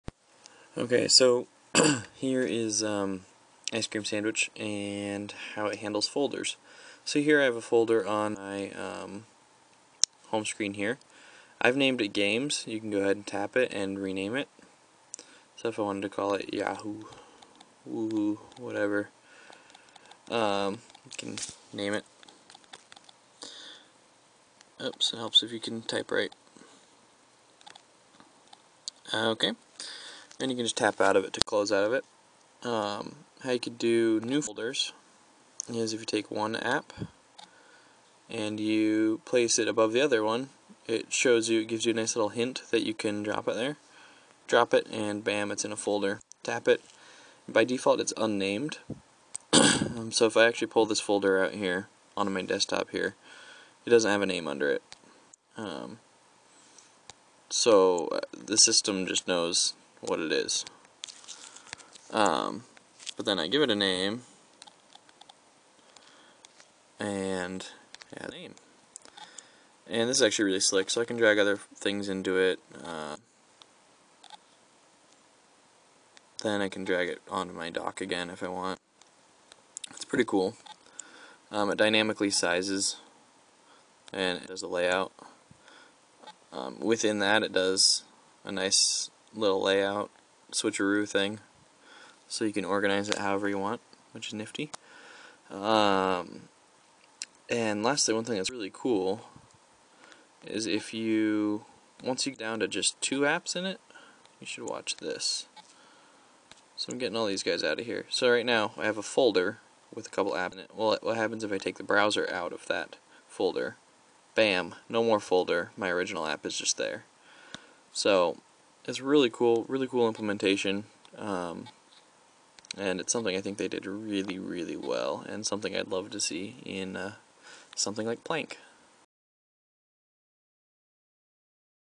Android 4.0 Folders
Just a quick screen cast demonstrating ICS folders.